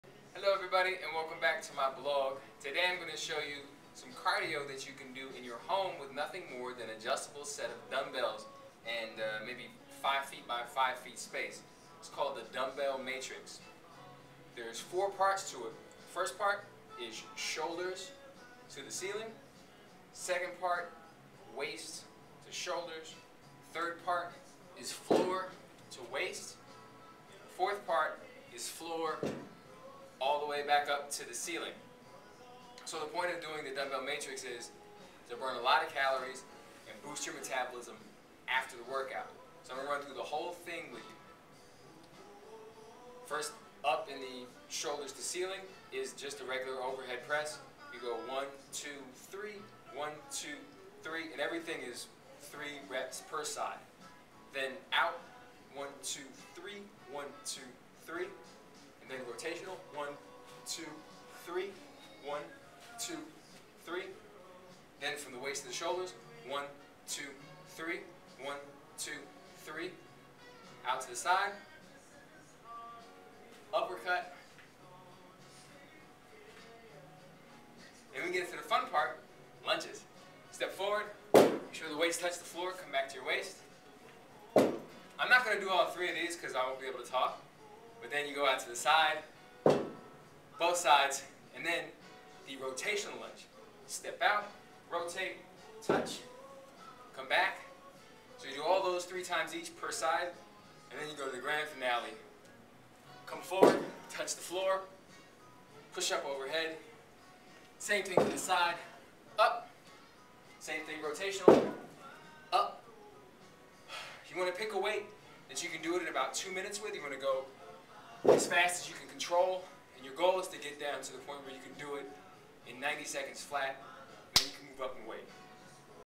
Washington DC Personal Trainer Shows You A Quick Home Fat Loss Blast
A FUN, quick and super-effective fat-loss interval workout that you can do at home with nothing more than a pair of dumbbells.  Josef Brandenburg is a 2009 Personal Trainer of the Year nominee, based in Washington, DC.